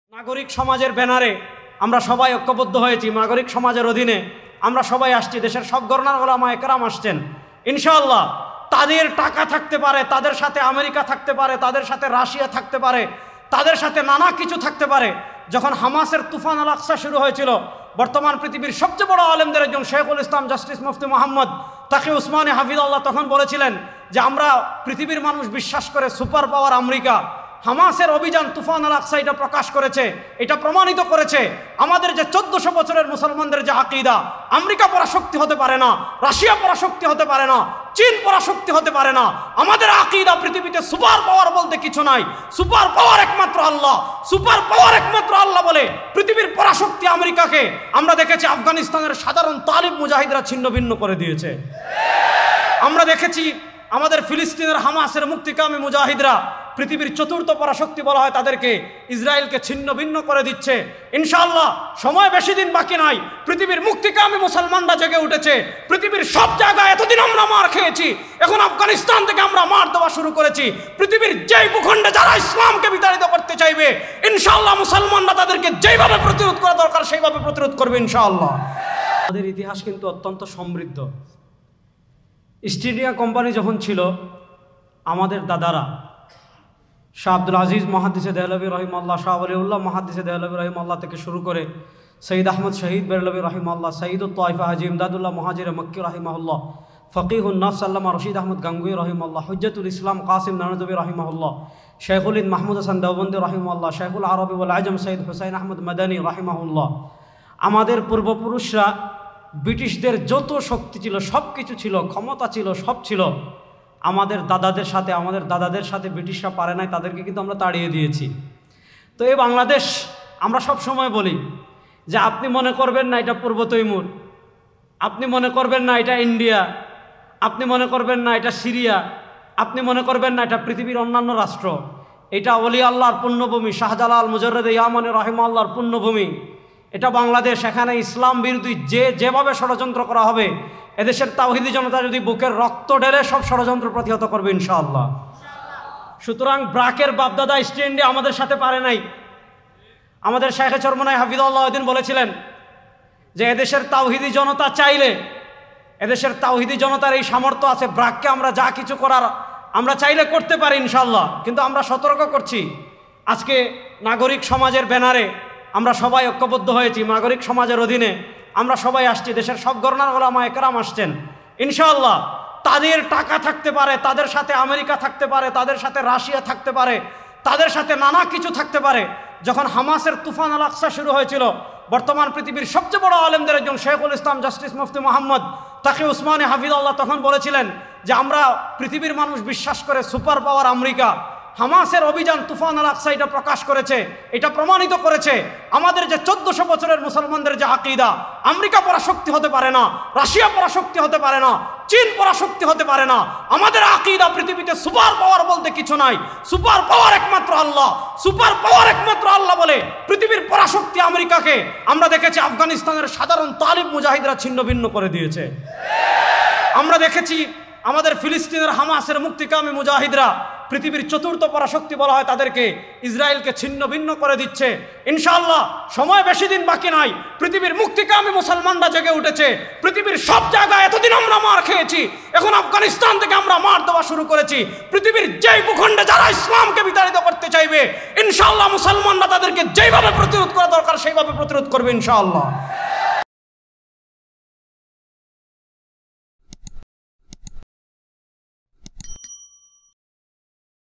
আজকের জাতীয় সেমিনারে সকল ঘরানার আলেমকে একত্রে করলেন রেজাউল করিম আবরার | Mufti Rezaul Karim Abrar
আজকের জাতীয় সেমিনারে সকল ঘরানার আলেমকে একত্রে করলেন মুফতি রেজাউল করিম আবরার | Mufti Rezaul Karim Abrar | R S Media

০৩ জুন ২০২৪ইং
জাতীয় সেমিনার
 ডিপ্লোমা ইঞ্জিনিয়ার ইনস্টিটিউশন, কাকরাইল, ঢাকা